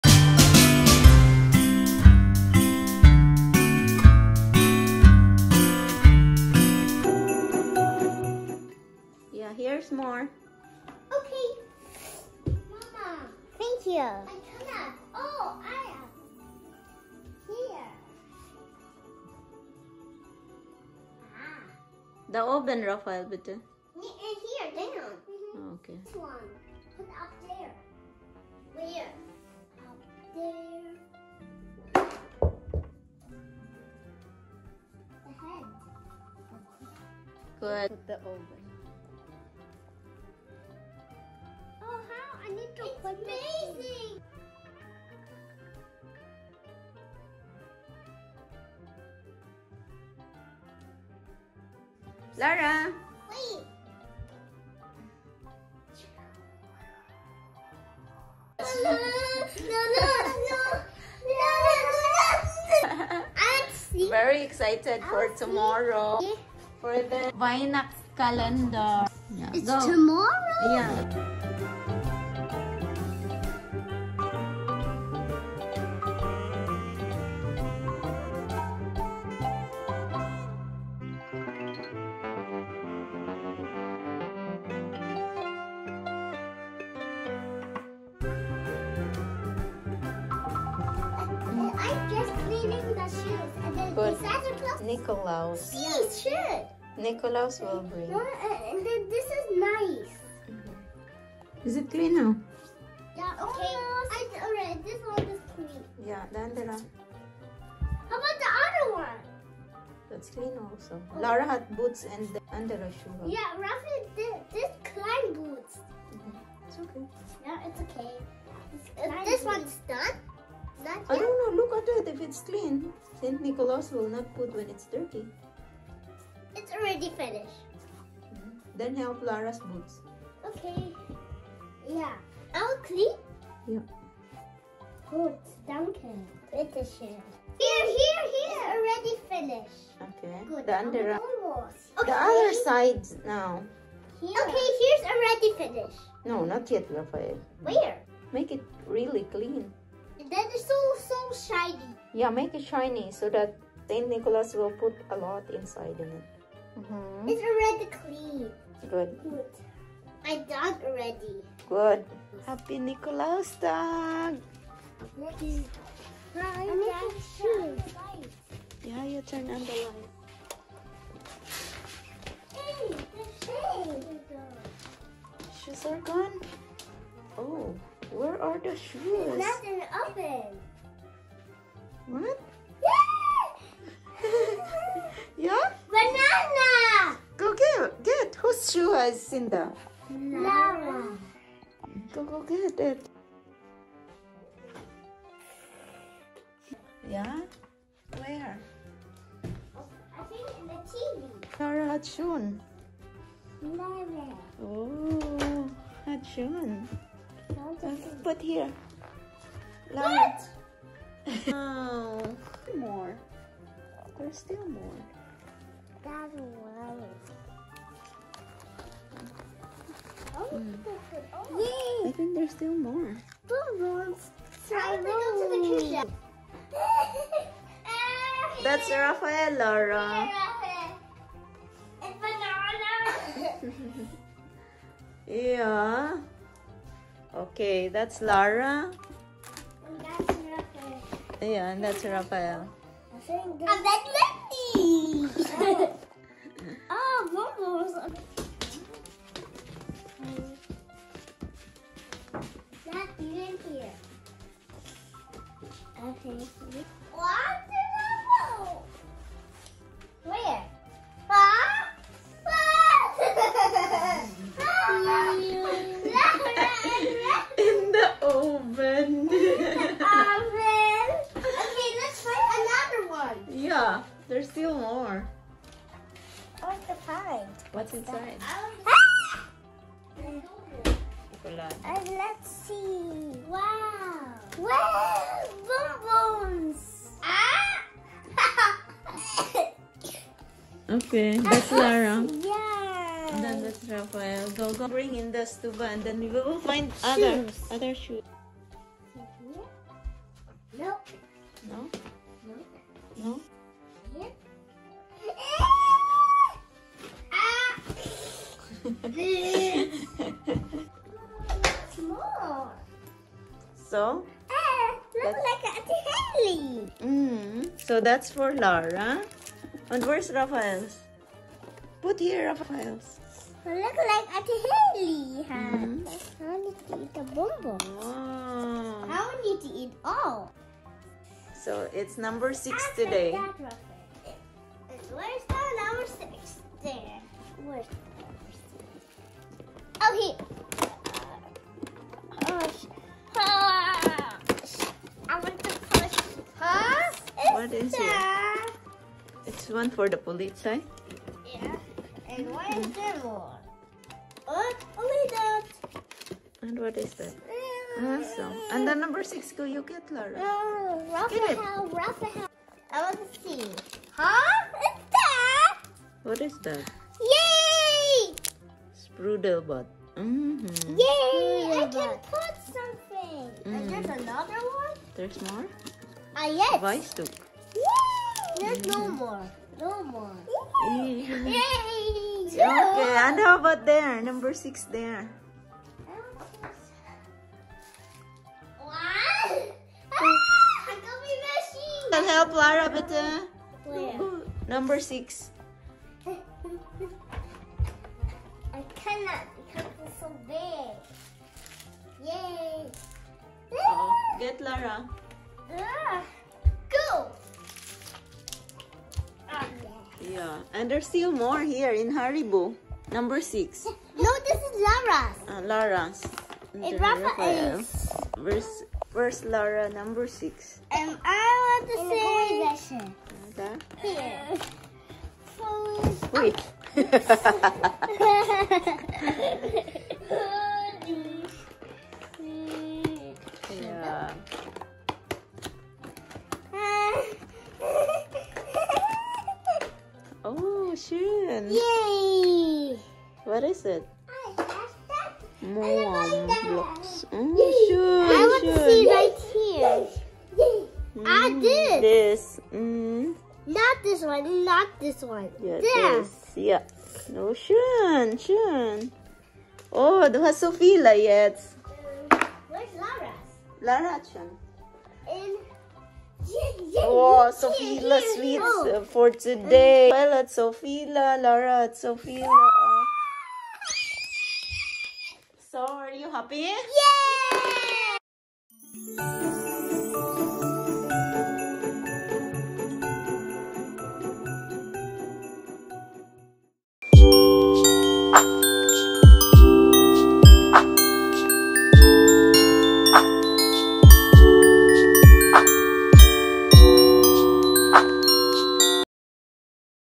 Adventszeit / Christmas Time/ St. Nicholas Day / Happy Kids / German Tradition
The magical time of the year -Christmas time! Enjoy the Yuletide Season everyone!🥰😘 The kids love to have deco and even did it by themselves. They are feel amaze of our preparations especially their Advent Calendars and they are eager to wake up early for it. They are even happier during St. Nicholas day knowing that there will be something in their shoes, they believed St. Nicholas put in them. A tradition in Germany - Nikolaustag.